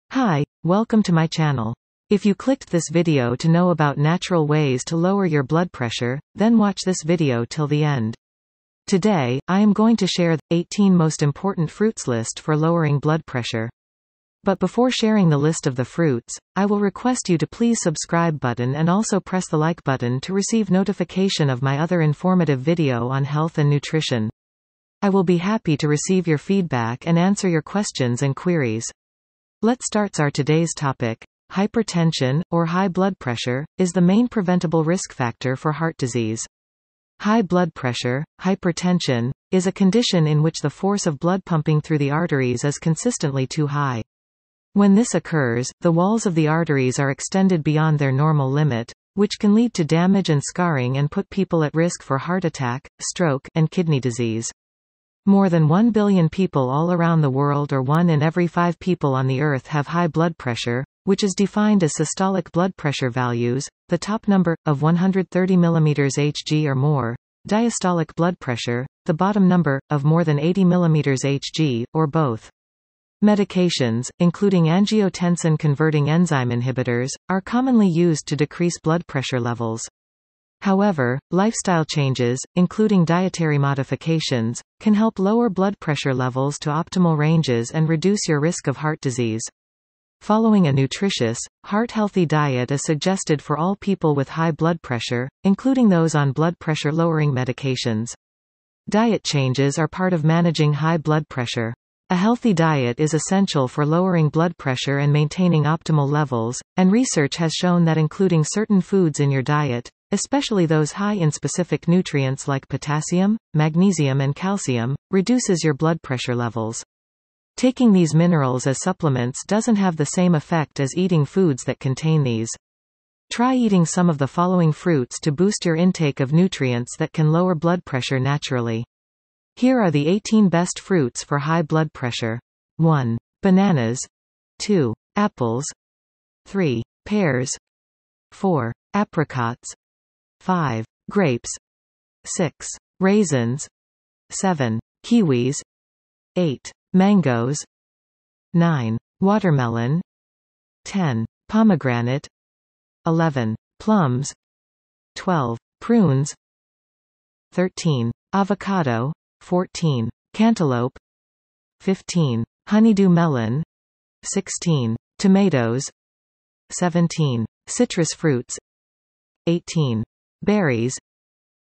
Best Fruits to Get Rid of High Blood Pressure and Lower blood Pressure (Hypertension)
This video contain a list of fruits and foods that are best for people with high blood pressure. these foods and fruits will help you to lower hypertension.